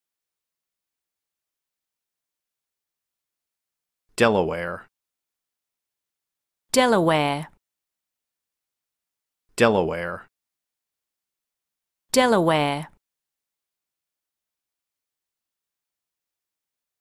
American vs Australian Accent: How to Pronounce DELAWARE in an Australian or American Accent
Today we are learning to pronounce the word DELAWARE